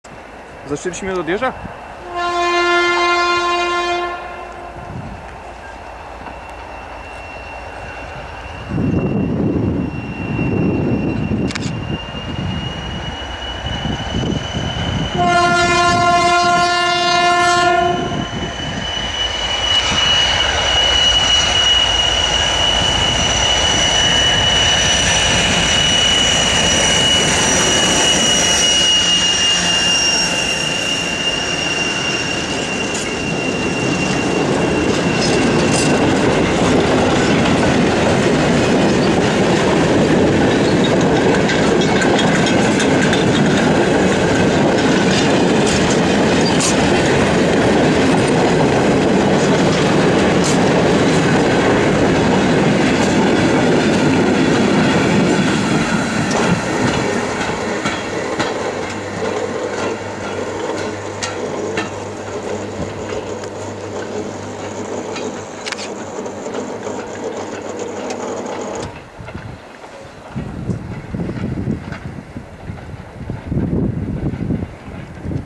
(22.02.13) TEM2-085 z "gruszkami" w Nieborowicach (+2xRp1)
Tamara z KP Kotlarnia przejeżdżająca przez podg. Nieborowice ze składem "gruszek" (13 4-osiowych serii Uacs i 4 2-osiowych serii Ucs) ze Szczygłowic w kierunku Kotlarni.
Pozdrowienia dla SewerynaET24 za wspólny wypad!